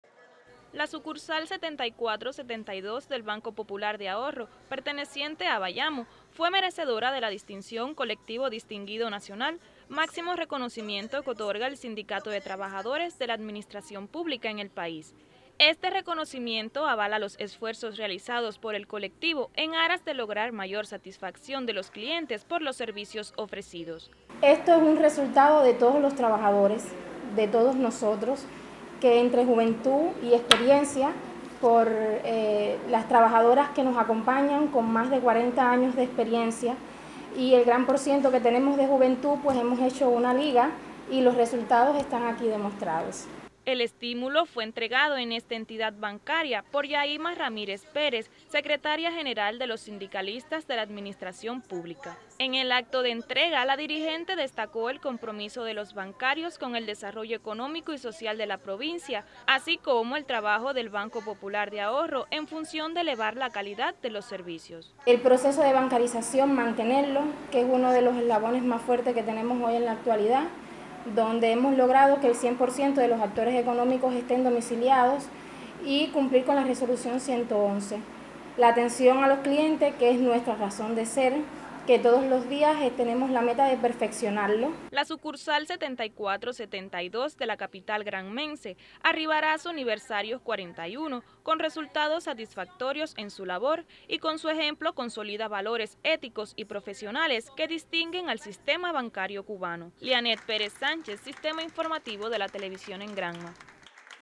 Trabajadores de la sucursal bancaria 7472 recibieron la condición de Colectivo Distinguido Nacional
El Banco Popular de Ahorro cumple 41 años de creado. Vísperas del aniversario, el colectivo de la sucursal 7472 ubicada en el paseo bayamés, recibió la condición de Colectivo Distinguido Nacional. Tal reconocimiento, otorgado por el Sindicato Nacional de Trabajadores de la Administración Pública,  avala el excelente trabajo realizado por quienes laboran en esa entidad.